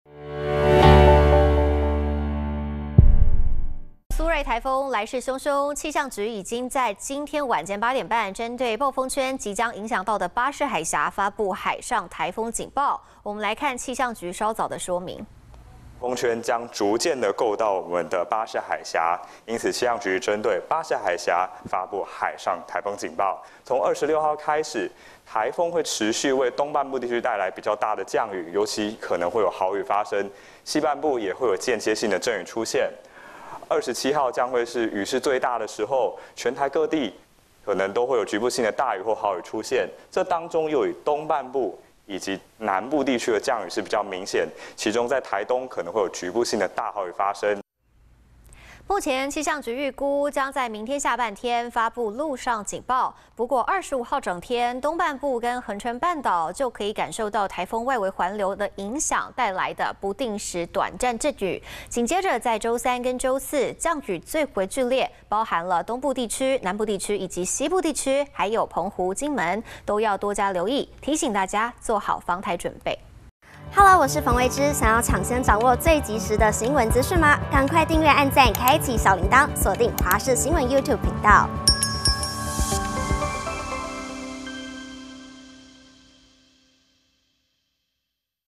「杜蘇芮」再長胖 氣象局估週二下半天發陸警｜華視新聞 20230725
今年第5號中度颱風「杜蘇芮」，來勢洶洶，氣象局已經在24日晚間八點半，針對暴風圈即將影響到的「巴士海峽」，發布「海上颱風警報」，來看氣象局的說明。